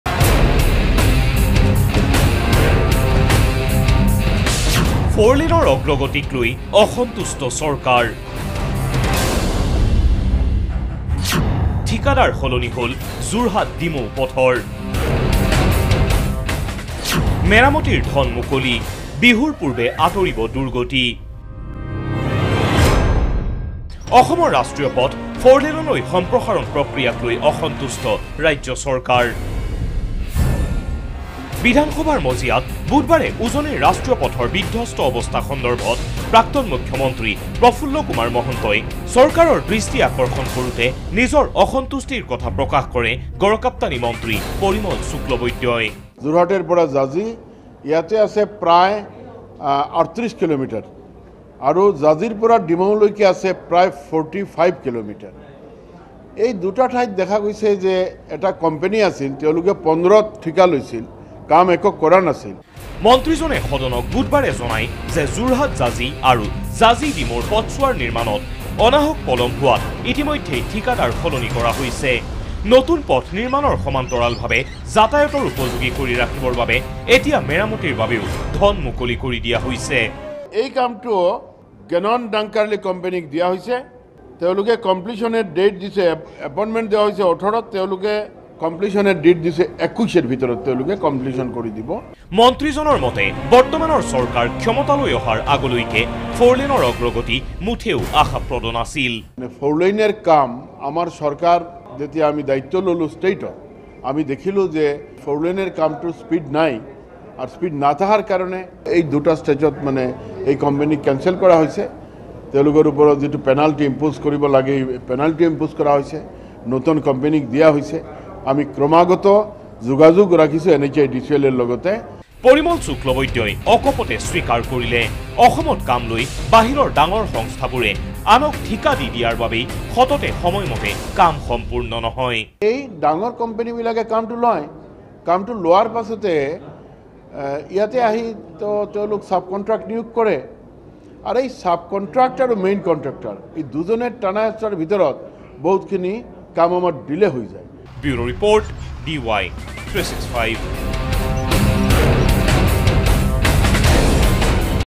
ফ’ৰ লেইনৰ অগ্ৰগতিক লৈ অসন্তুষ্ট চৰকাৰ || Slow progress of four-lane
অসমৰ ৰাষ্ট্ৰীয় পথ ফৰলেনলৈ সম্প্ৰসাৰণ প্ৰক্ৰিয়াক লৈ অসন্তুষ্ট ৰাজ্য চৰকাৰ। বিধান সভাৰ মজিয়াত বুধবাৰে উজনিৰ ৰাষ্ট্রীয় পথৰ বিধ্বস্থ অৱস্থা সন্দৰ্ভত প্ৰাক্তন মুখ্যমন্ত্ৰী প্ৰফুল্ল কুমাৰ মহন্তই চৰকাৰৰ দৃষ্টি আকৰ্ষণ কৰোতে নিজৰ অসন্তুষ্টিৰ কথা প্ৰকাশ কৰে গড়কাপ্টানী মন্ত্ৰী পৰিমল শুক্লবৈদ্যই।